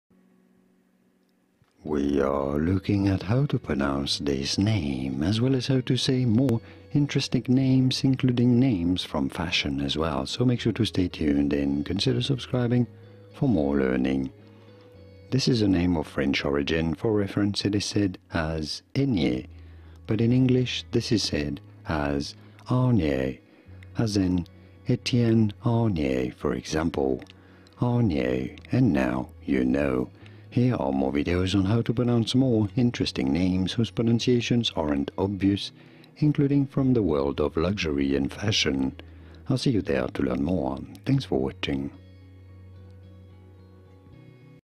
How to Pronounce Aigner? (CORRECTLY)
This video shows you how to pronounce Aigner (Etienne, American Fashion Designer, brand, name, company, pronunciation guide).

Information & Source
Étienne Aigner was the founder of Etienne Aigner US and Etienne Aigner AG, two presently separate high fashion houses, based respectively in New York City and Munich, Germany. Both companies produce luxury goods including handbags, shoes, women's ready-to-wear, wallets, and leather accessories. Wikipedia Source
Born: 1904, Nové Zámky, Slovakia
Died: 2000, New York, New York, United States
Full name: Étienne Aigner
Spouse: Suzanne Aigner (m. 1938–2000)
Children: Patricia Bonnet
Siblings: Lucien Aigner
People also search for
Sonia Rykiel
Christian Louboutin
Giorgio Armani
Roberto Cavalli
Alexander McQueen